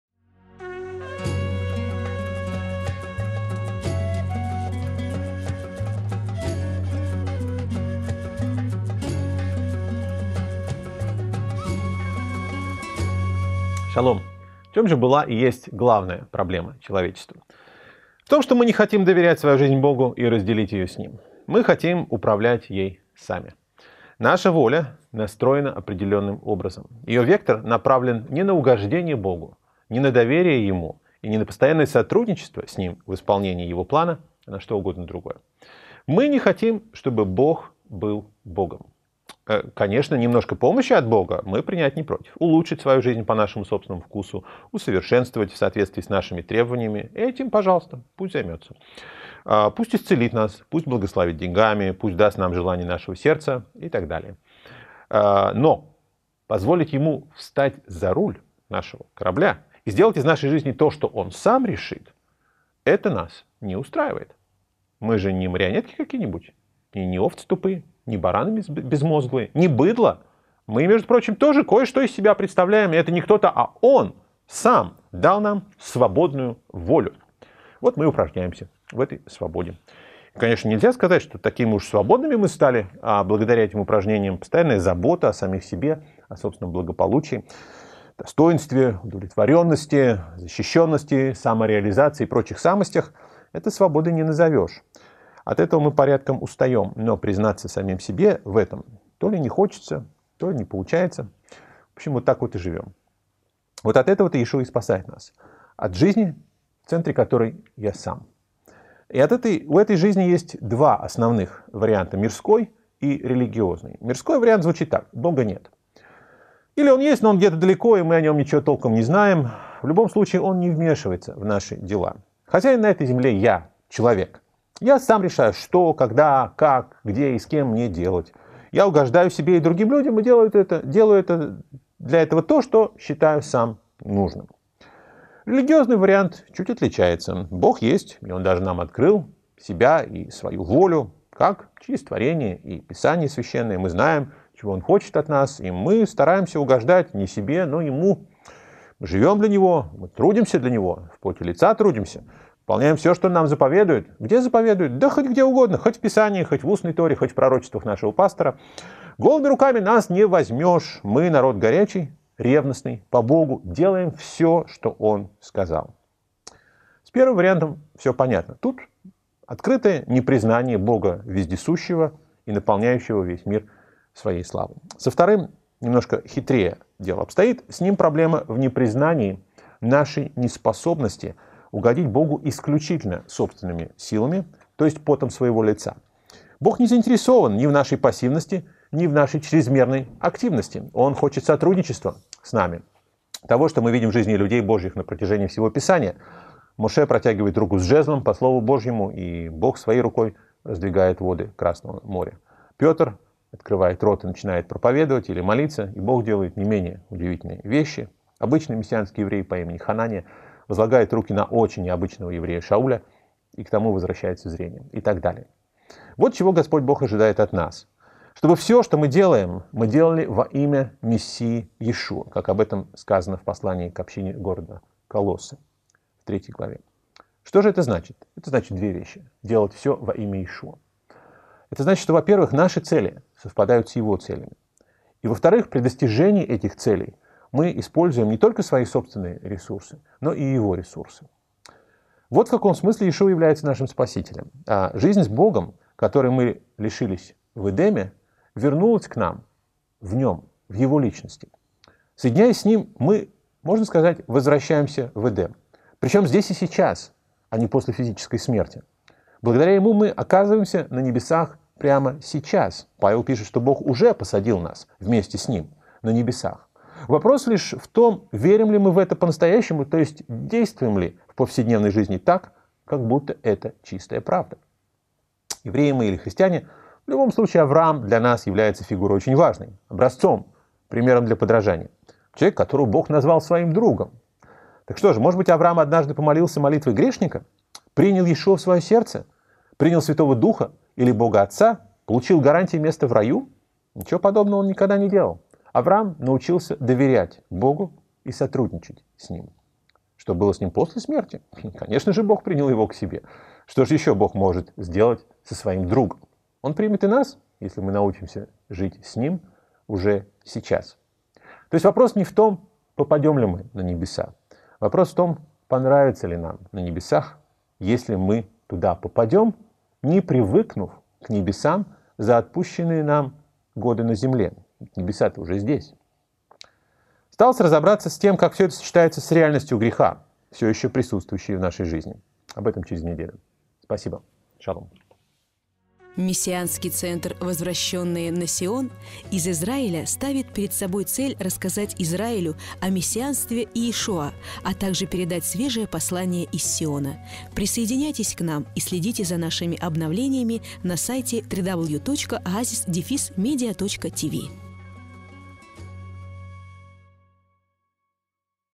О спасении /сколько можно грешить. Часть 4 (А. Гольдберг)
Что делать, что бы спастись?! Что значит "Иешуа-Спаситель"?! От чего нам необходимо спасение?! Приглашаем вас продолжить изучение этих и других важных вопросов в серии минипроповедей "О спасении" Александра Гольдберга